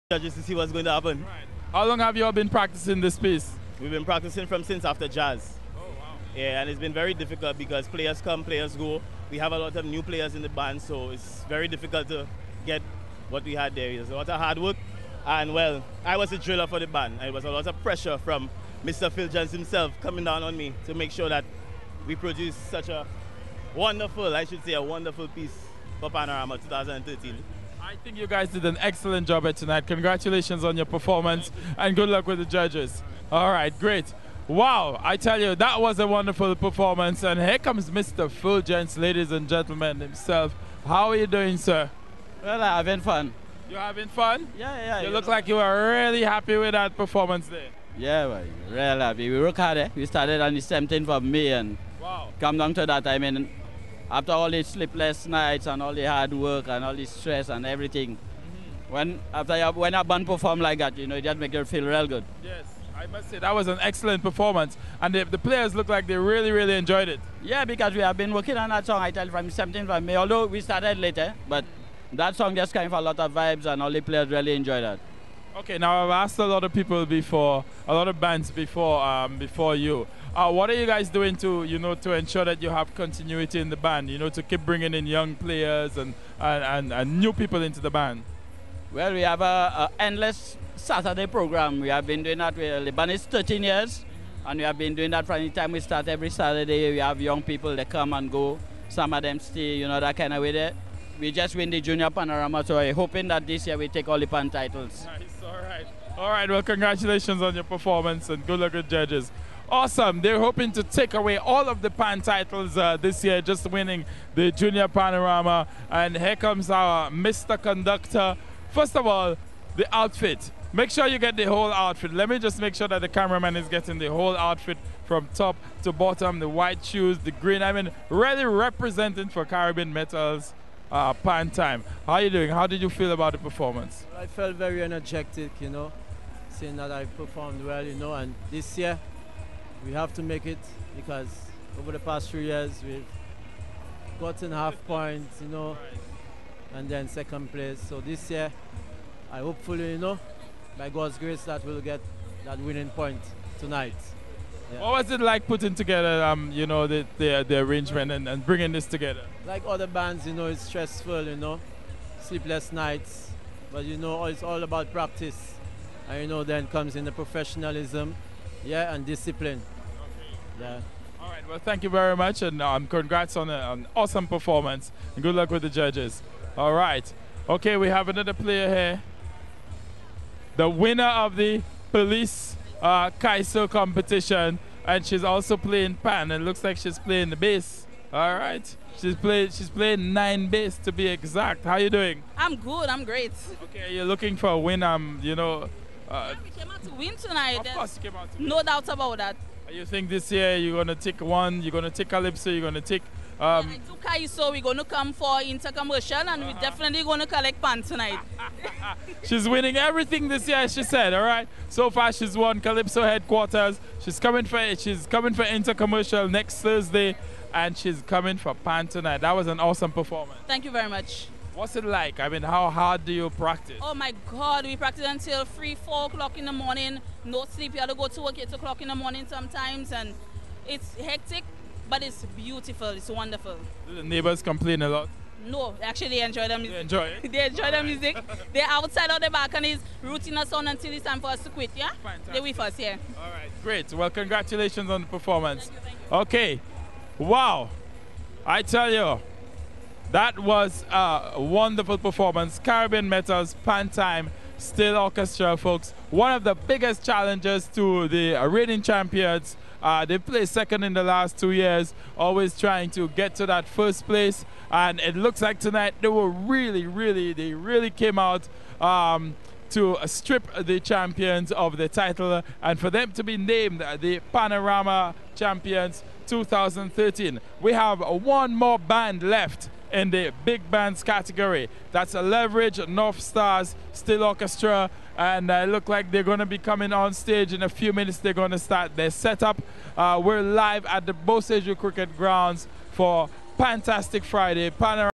Caribbean Metals Pantime - Interviews
Interviews conducted after Pantime's Winning Performance at the 2013 St. Lucia Panorama